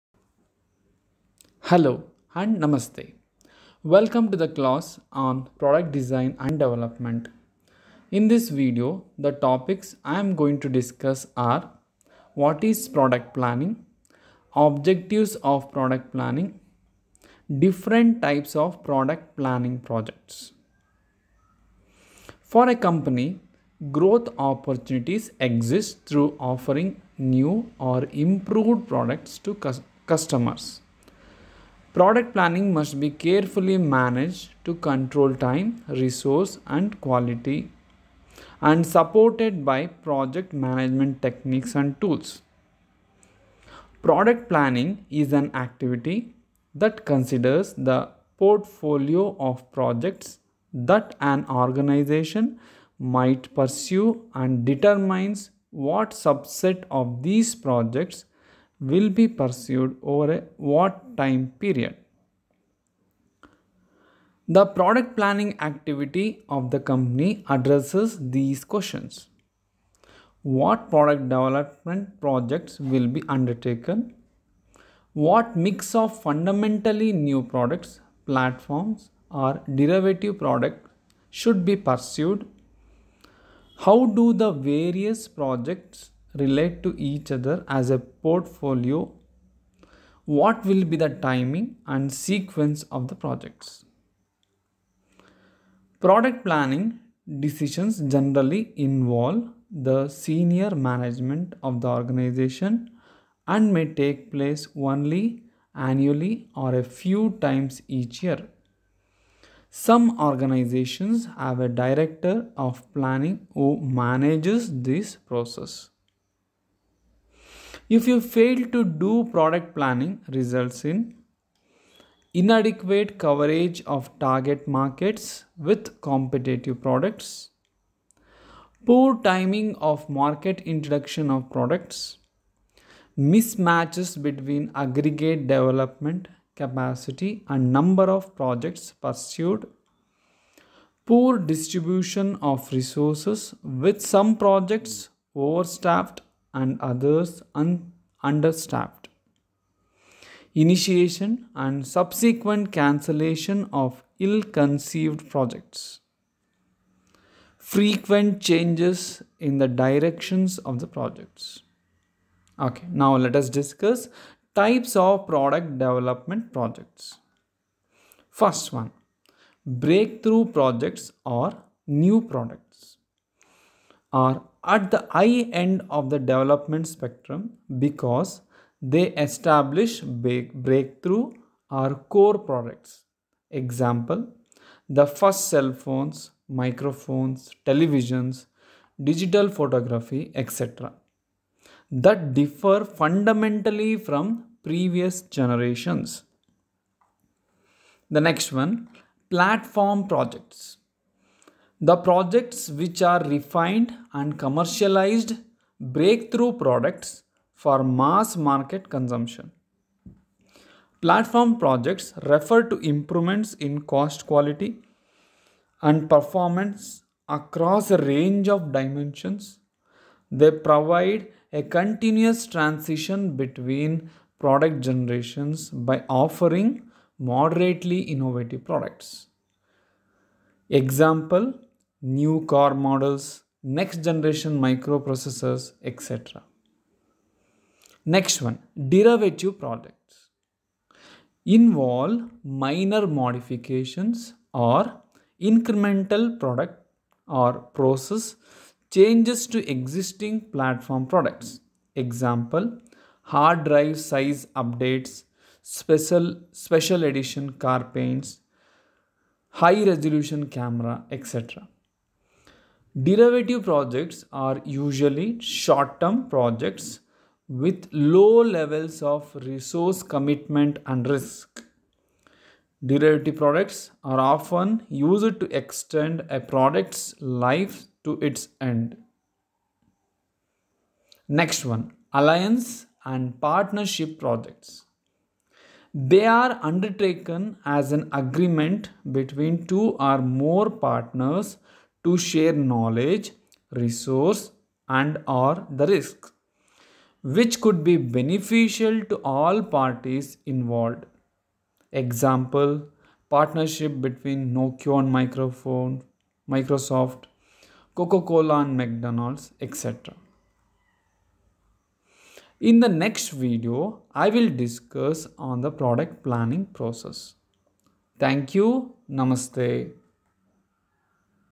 Product Planning/Product Design & Development/Unit -2
Product Planning/Product Design & Development

• Product planning is an activity that considers the portfolio of projects that an organization might pursue and determines what subset of these projects will be pursued over what time period.